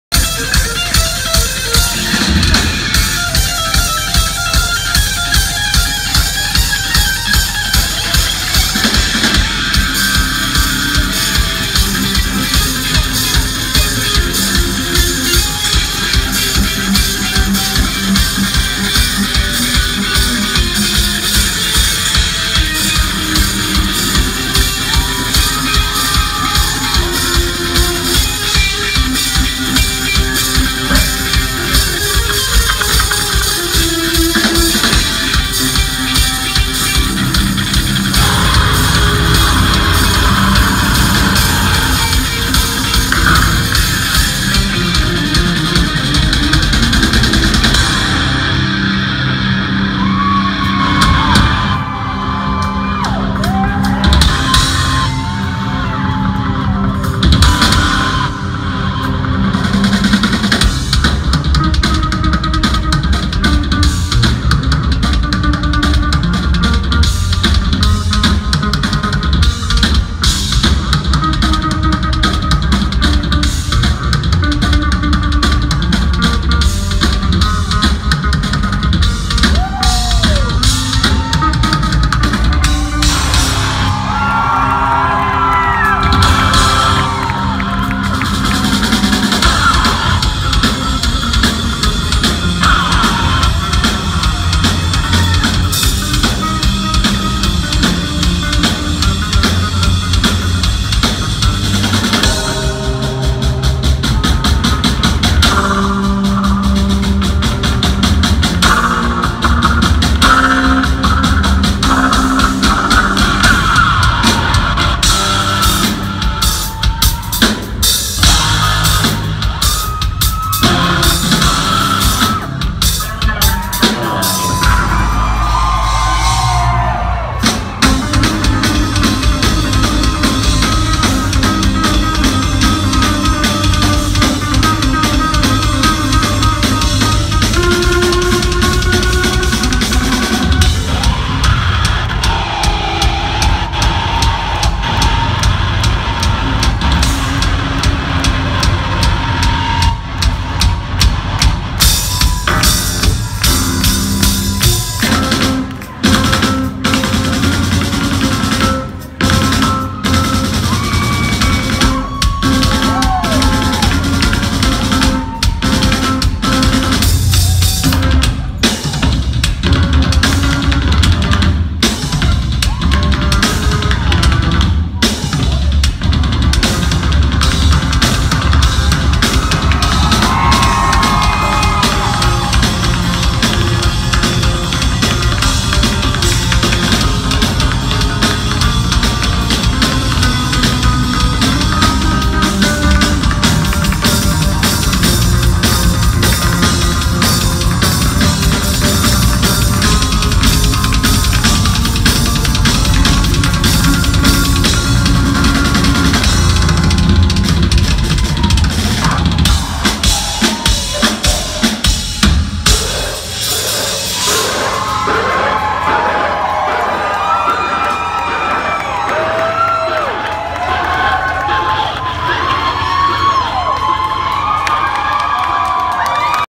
Nothing More. Bass Solo
Bass Solo by Nothing More live at The International Knoxville TN 9-21-16